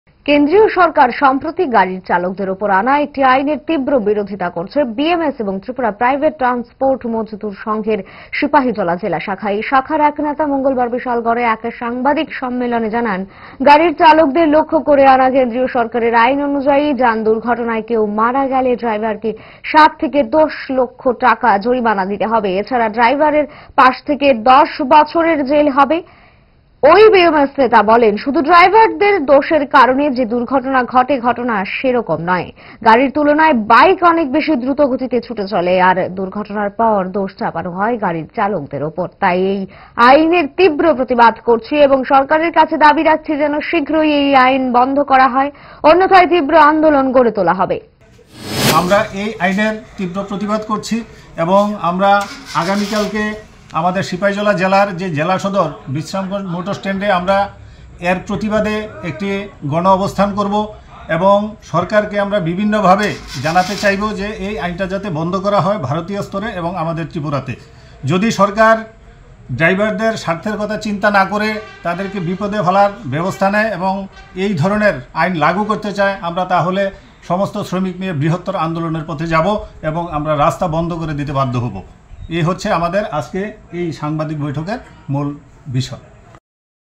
কেন্দ্রীয় আইন বাতিলের দাবিতে রাজ্যে সরব হয়েছে বিএমএস।
গাড়ি চালকদের উপর কেন্দ্রীয় সরকার সম্প্রতি আনা একটি আইনের তীব্র বিরোধিতা করেছে BMS এবং ত্রিপুরা প্রাইভেট ট্রান্সপোর্ট মজদুর সংঘের সিপাহীজলা জেলা শাখা।